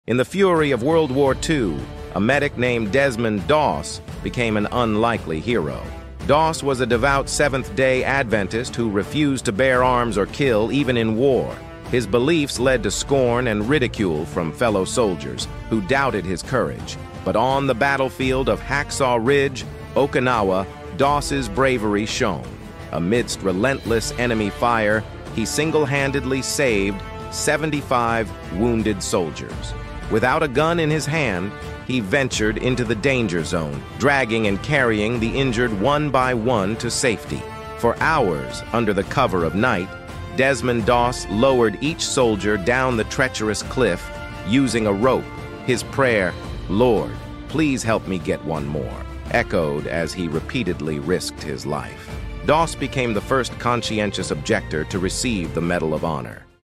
Desmond Doss, The Unarmed Hero of Hacksaw Ridge.
In the fury of World War II, a medic named Desmond Doss became an unlikely hero. Doss was a devout Seventh-day Adventist who refused to bear arms or kill, even in war. His beliefs led to scorn and ridicule from fellow soldiers, who doubted his courage. He single-handedly saved 75 wounded soldiers.